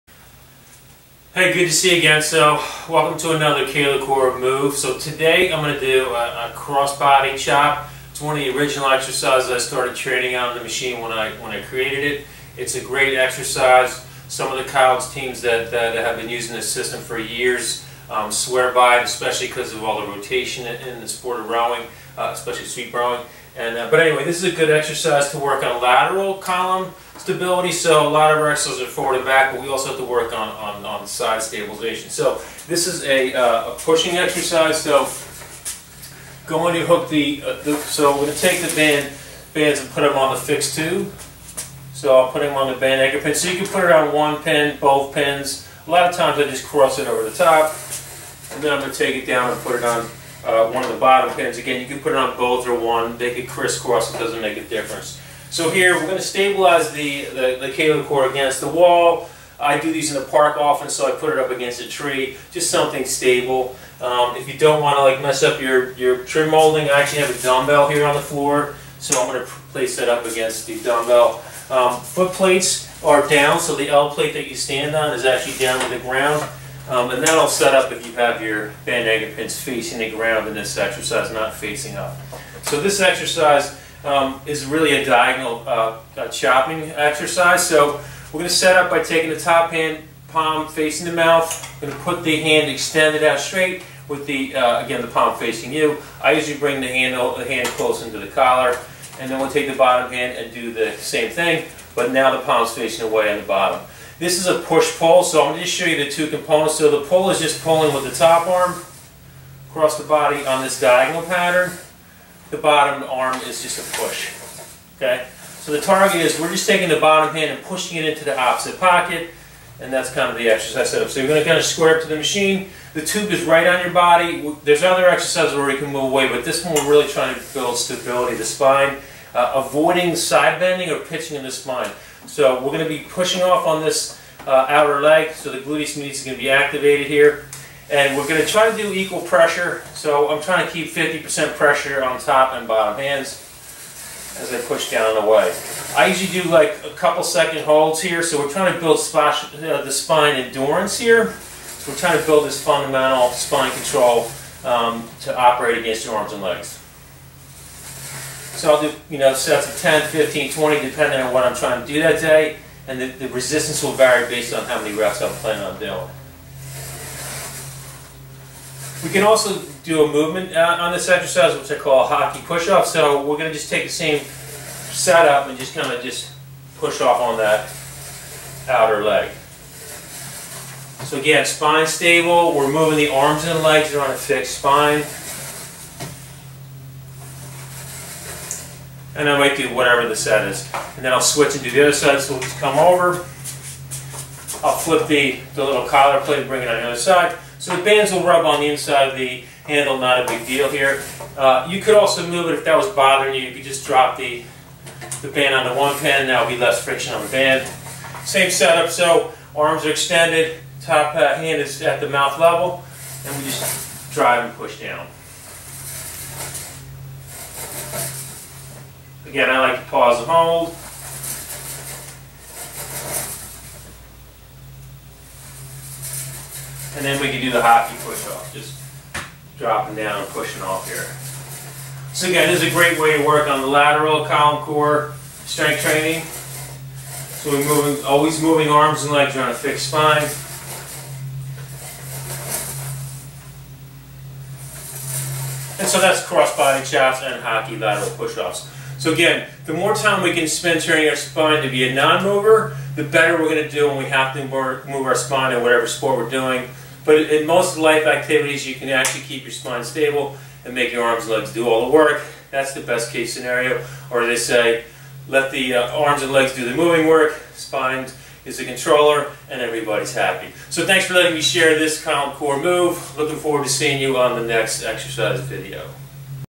Cross-body Chops
This exercise focuses on build lateral column core spine control using diagonal chops to build maximum strength in the arms while building a stable trunk for the arms to work against.